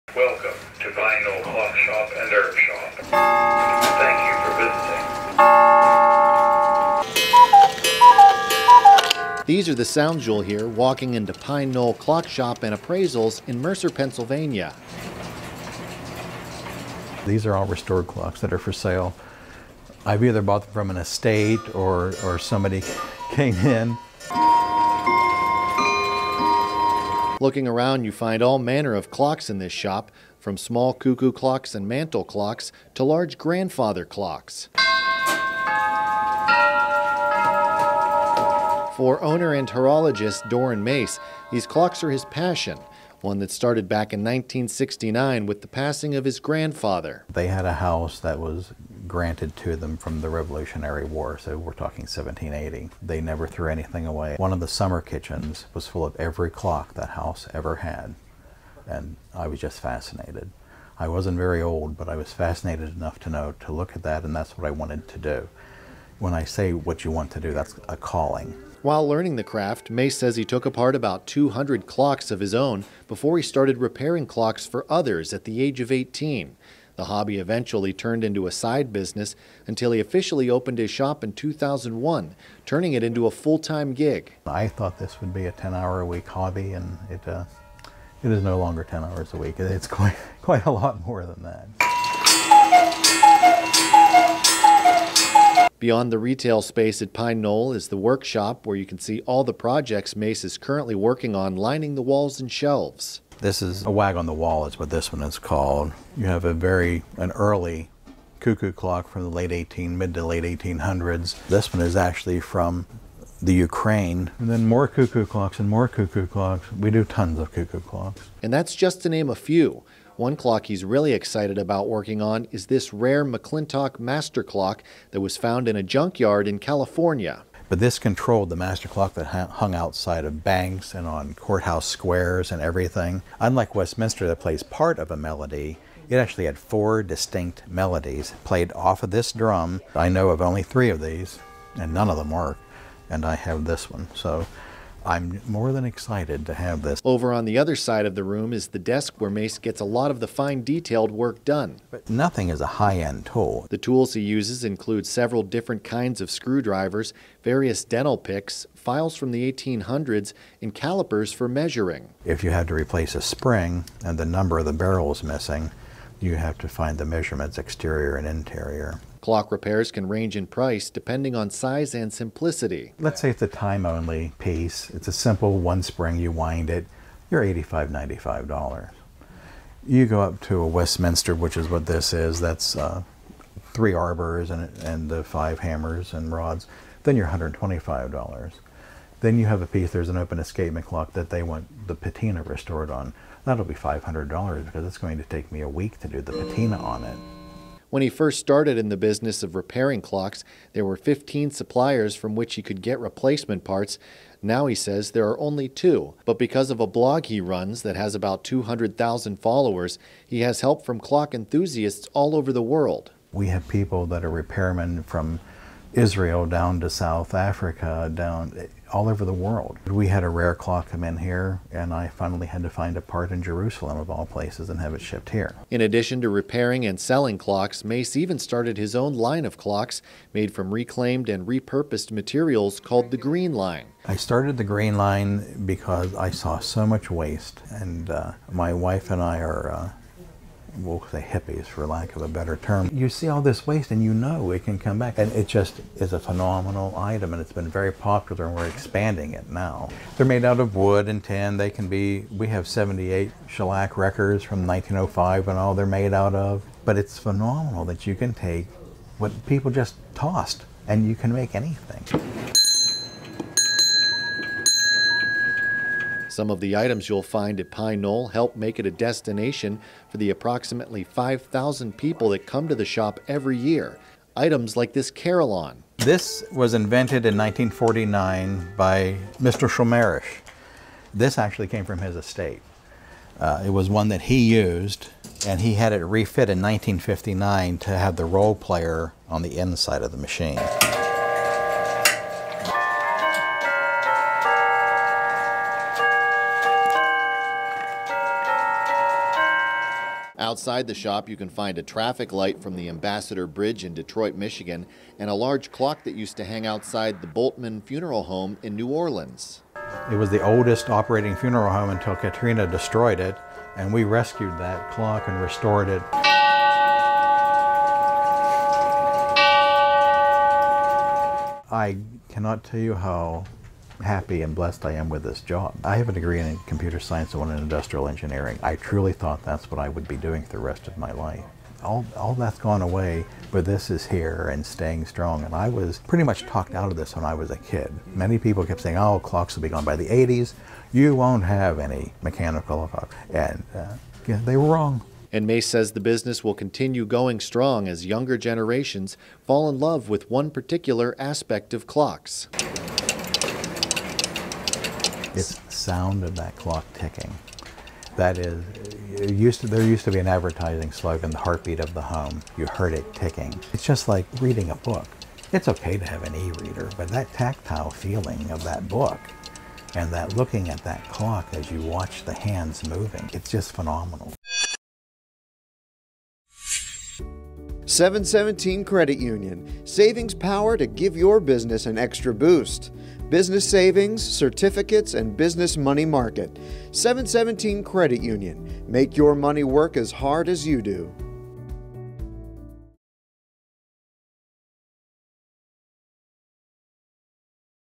Inside the World of Clocks
At Pine Knoll Clock Shop, owner Dorrin Mace keeps decades of history ticking along.